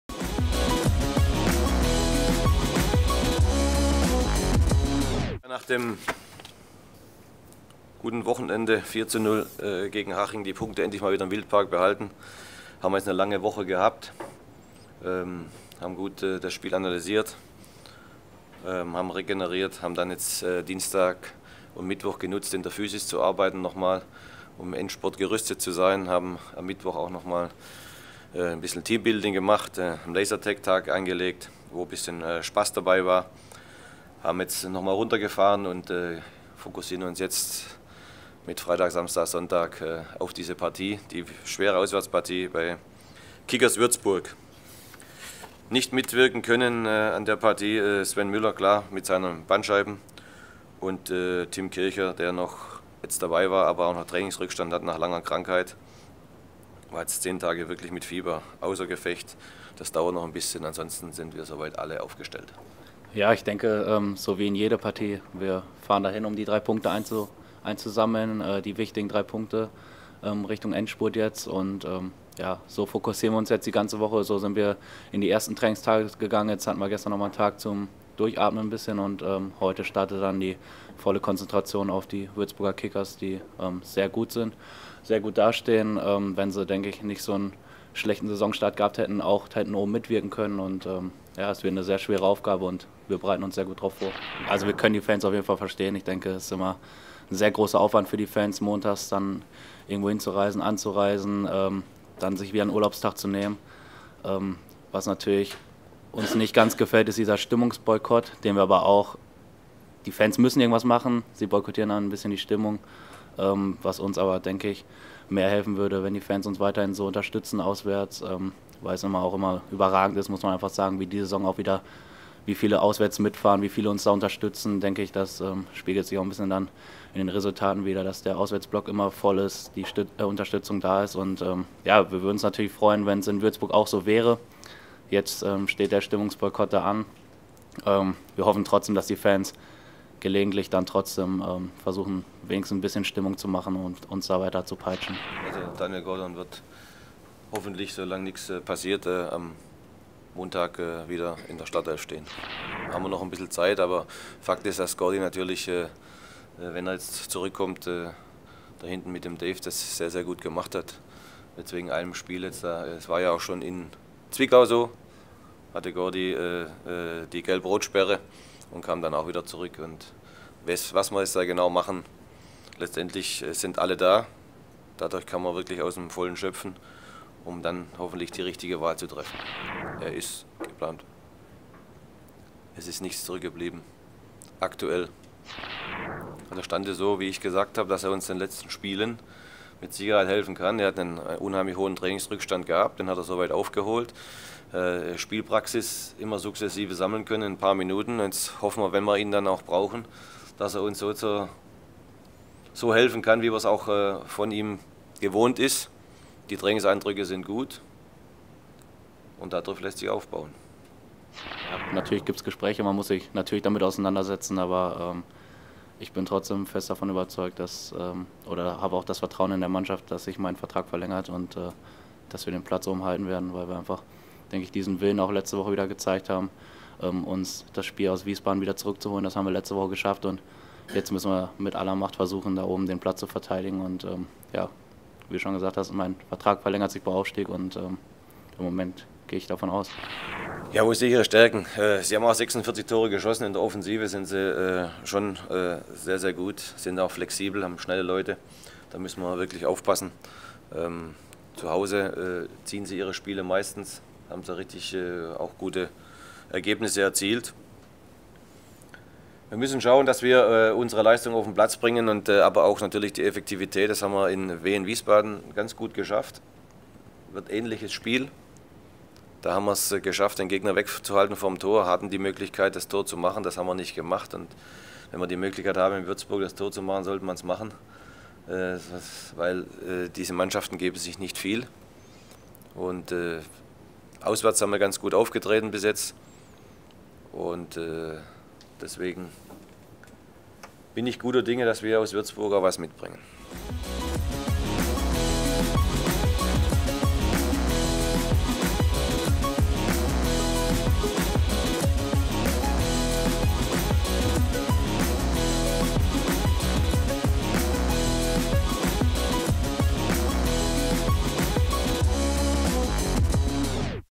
KSC-Pressekonferenz vor Würzburg (33. Spieltag)
Cheftrainer Alois Schwartz und Marc Lorenz blicken auf unseren nächsten Gegner. Am Montag (Anpfiff: 19 Uhr) geht es in der Flyeralarm Arena gegen die Würzburger Kickers.